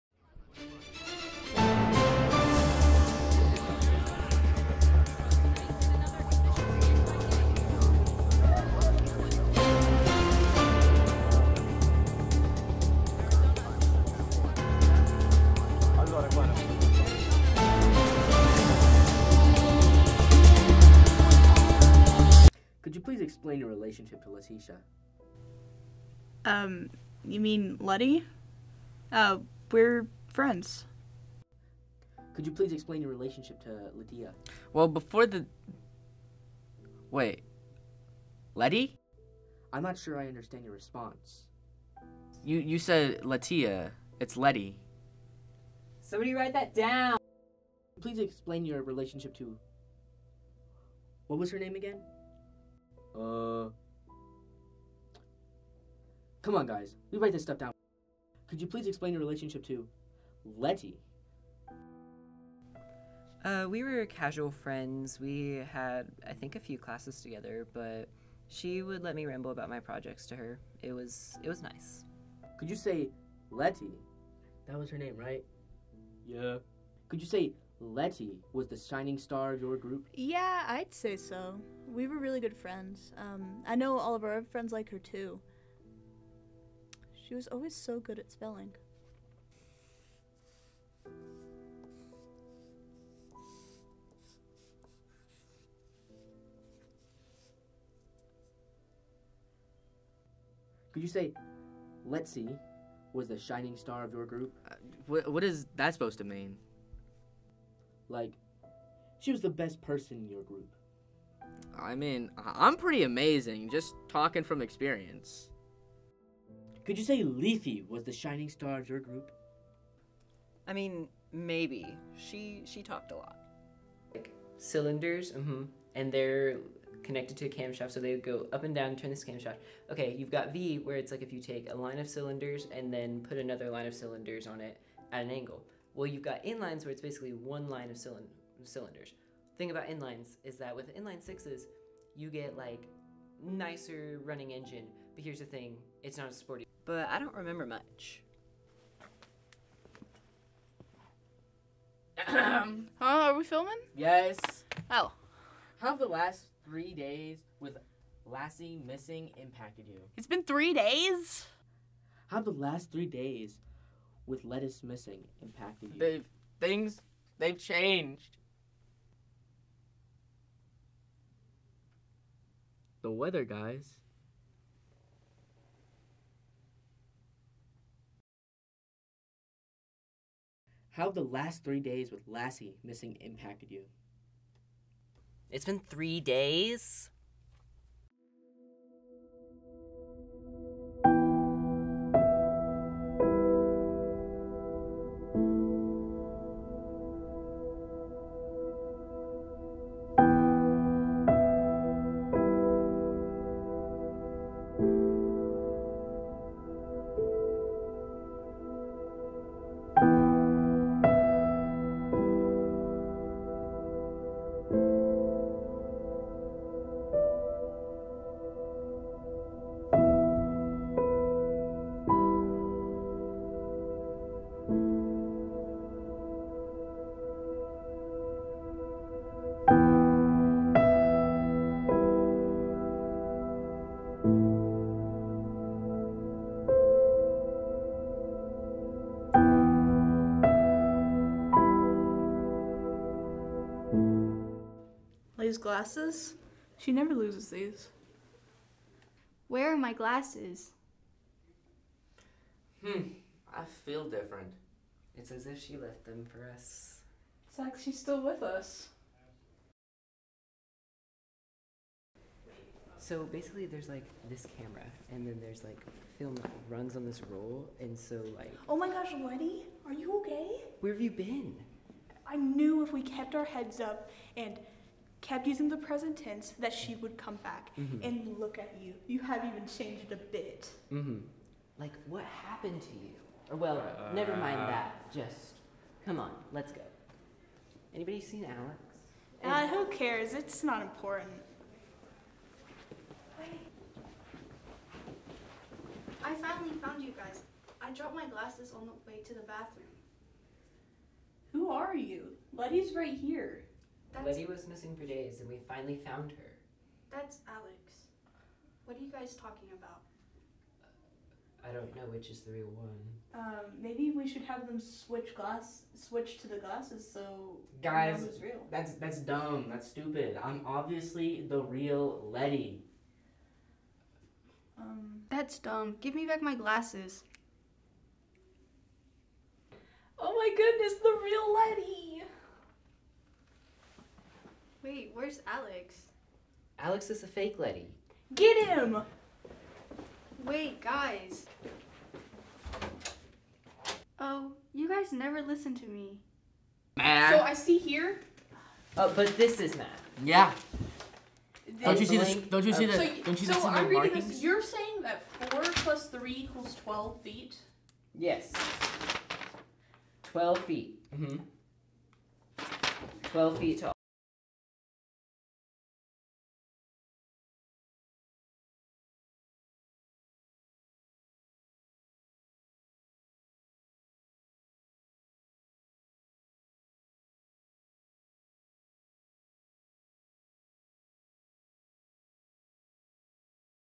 1-800-LETY
Lety goes missing and a group of students look for her... they do their best her but their looking in the wrong places.